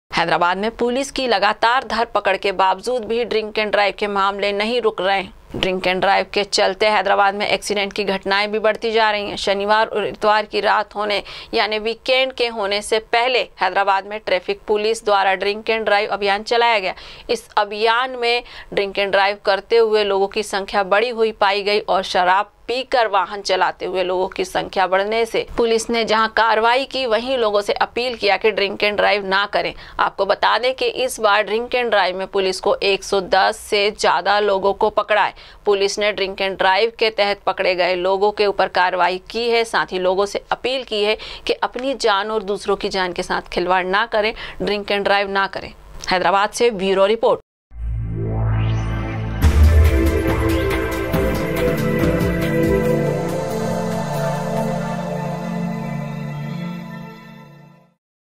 [Hyderabad News]हैदराबाद में पुलिस की लगातार धरपकड़ के बावजूद भी ड्रिंक एंड ड्राइव
हैदराबाद में पुलिस की लगातार धरपकड़ के बावजूद भी ड्रिंक एंड ड्राइव के मामले नहीं रुक रहे हैं ट्रेन के ड्राइवर के चलते हैदराबाद में एक्सीडेंट की घटनाएं भी बढ़ती जा रही है शनिवार इतवार की रात होने से पहले वीकेंड से पहले हैदराबाद ट्रैफिक पुलिस द्वारा ड्रिंक एंड राय अभियान चलाया गया था